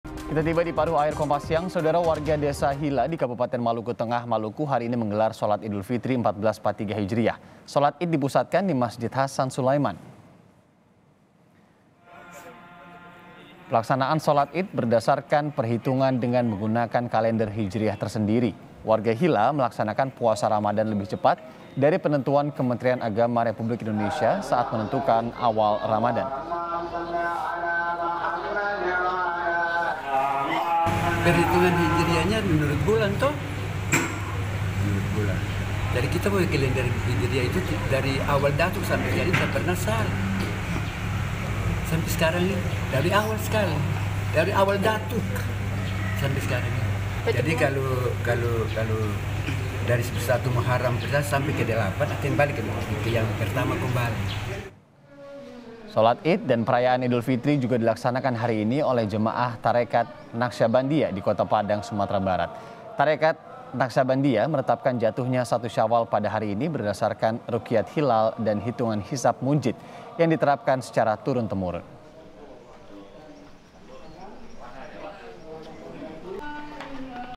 Yakini Hitungan Tersendiri, Warga Desa Hila Maluku Gelar Salat Idul Fitri 1443 H Hari Ini
MALUKU, KOMPAS.TV - Warga Desa Hila, di Kabupaten Maluku Tengah, Maluku, hari ini (01/05) menggelar Sholat Idul Fitri 1443 hijriah.

Sholat Id dipusatkan di Masjid Hasan Sulaiman.

Pelaksanaan sholat id berdasarkan perhitungan dengan menggunakan kalender hijriah tersendiri.

Baca Juga Pedagang Kue Kering Musiman Keluhkan Penurunan Pembeli di Lebaran 2022 di

Warga Hila melaksanakan puasa ramadan lebih cepat, dari penentuan Kementerian Agama, saat menentukan awal ramadan.

Sholat id, dan perayaan idul fitri, juga dilaksanakan hari ini (01/05), oleh Jemaah Tarekat Naqsabandiyah, di Kota Padang, Sumatera Barat.

Tarekat Naqsabandiyah menetapkan jatuhnya satu syawal pada hari ini, berdasarkan rukiat hilal dan hitungan hisab munjid, yang diterapkan secara turun temurun.